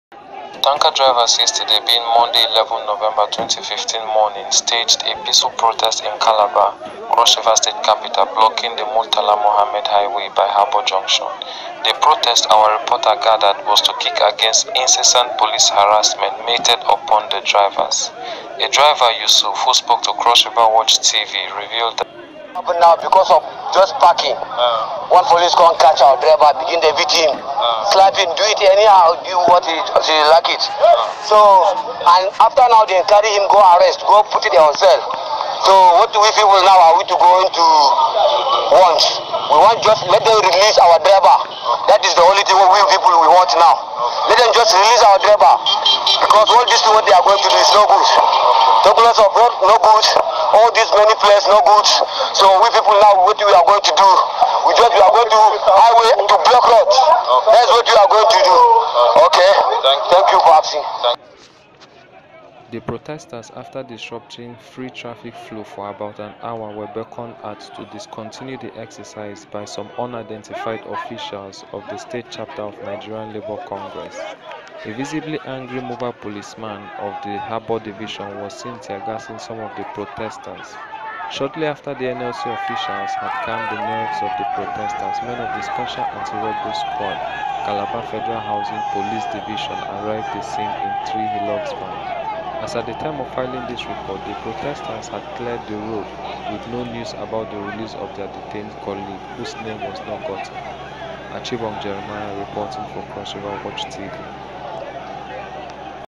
CrossRiverWatch TV
CALABAR: Tanker drivers protest police harassment, blocks highway.